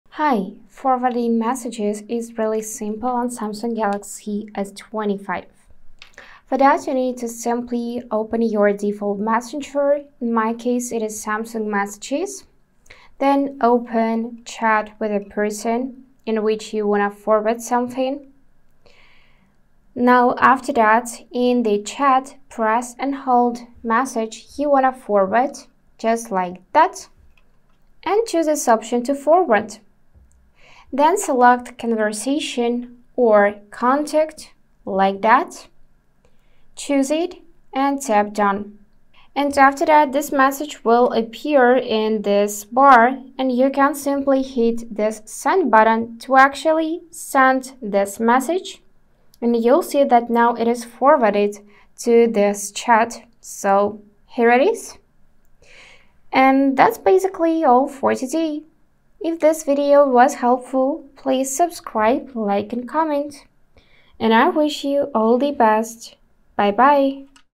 Samsung Galaxy S25 - How to Forward Text Messages
Discover the easiest way to forward messages on your new Samsung Galaxy S25! This guide will show you step-by-step how to quickly share conversations with multiple contacts all at once, straight from your device's messaging app. Whether it's an important update or a fun chat you want to share with friends or family, I'll help you master this useful feature in no time. Stay until the end for some additional tips and tricks that make messaging on the S25 a breeze.

This video also covers the following topics:
How to Share Texts on Samsung Galaxy S25
Samsung Galaxy S25 Guide: Forwarding a Message
Forwarding Messages on Samsung Galaxy S25: A Simple How-To